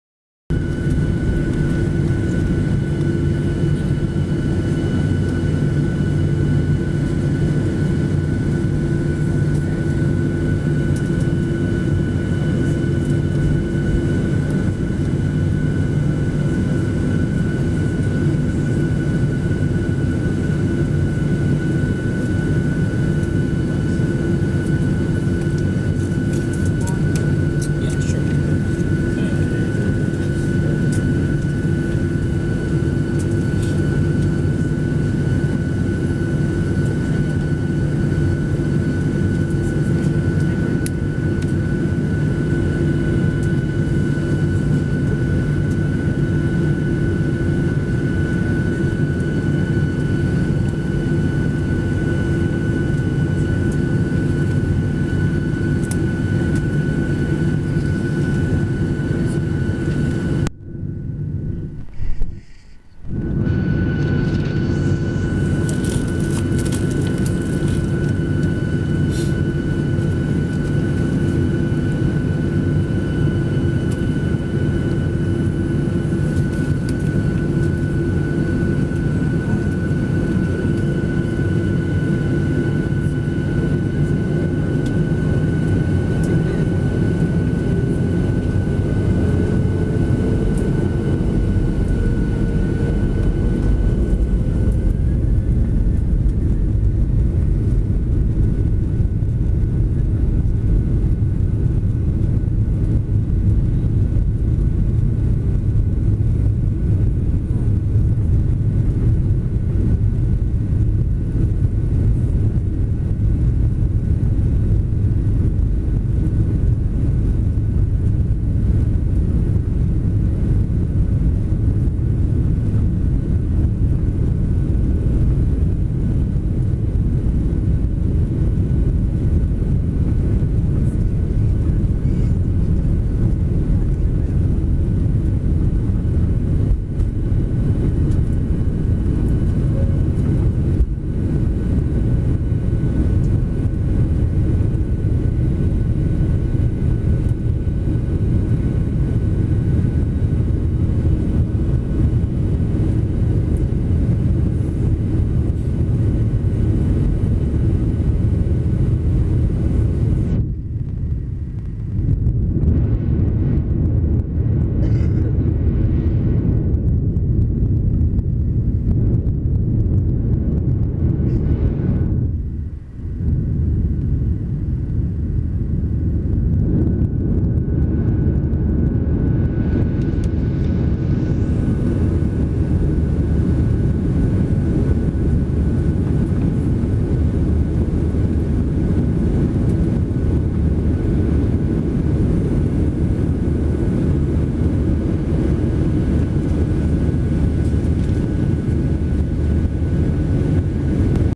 Flying out of Barbados heading to Jamaica!!
Travelling and leaving Barbados on my way to Jamaica for the first time.It was a nice clear day to fly.God is Good.